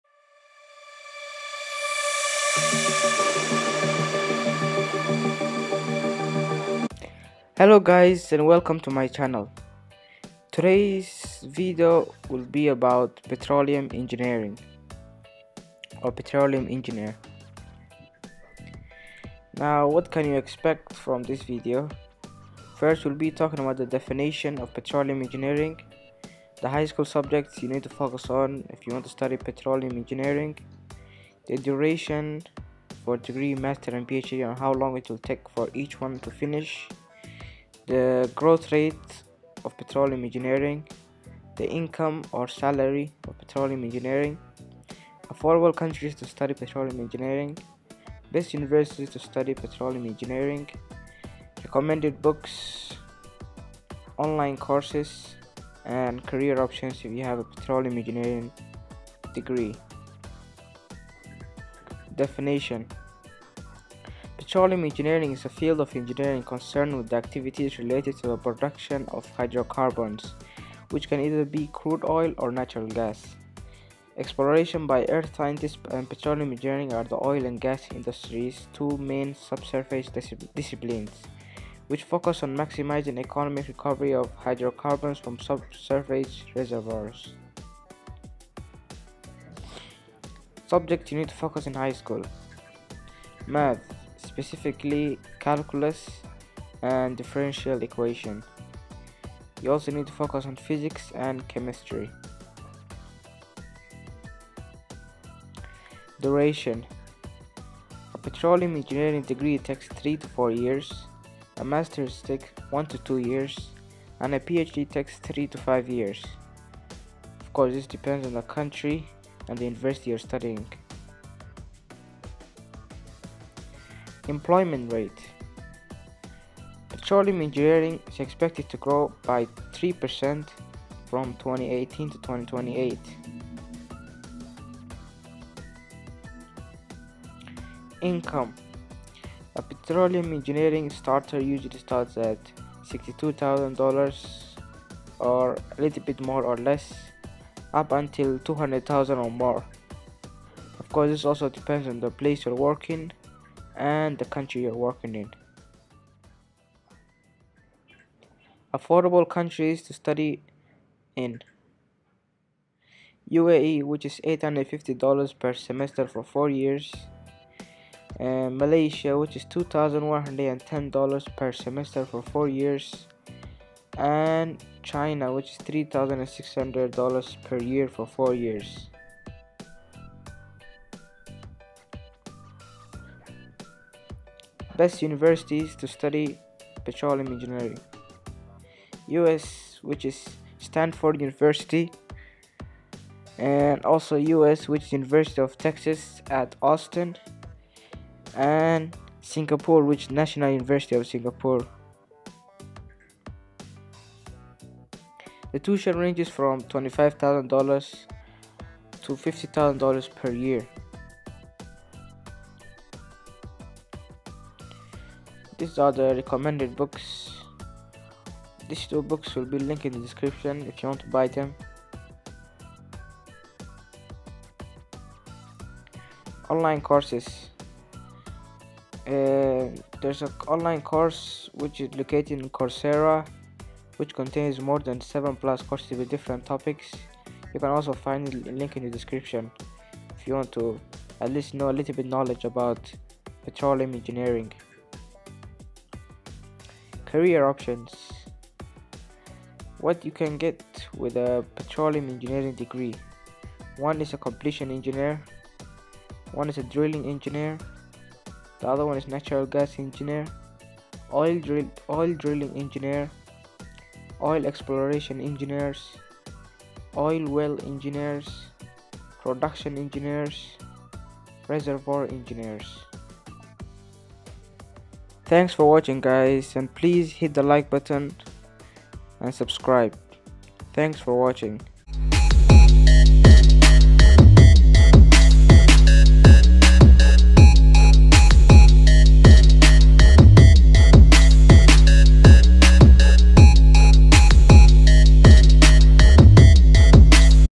Beginner's Guide to Petroleum Engineer; Salary, Jobs and Skills Simple
This videos is about Petroleum Engineering details that you need to know before choosing or majoring in it. This includes income, employment rate and many more.This video is made for mostly students in high school who want to major Petroleum Engineering.

VIDEO TIMELINE:
00:00- Intro
00:18- What To Expect
01:00- Definition
01:29-  Subjects You Need To Focus In High School
01:44- Duration
02:03- Employment Rate
02:16- Income
02:37- Affordable Countries To Study In
03:03- Best Universities To Study In
03:34- Books
03:45- Online Courses
04:05- Career Options
04:41- outro

- 1st book (Oil 101)

- 2nd book (Practical Optimisation of Petroleum Production Systems)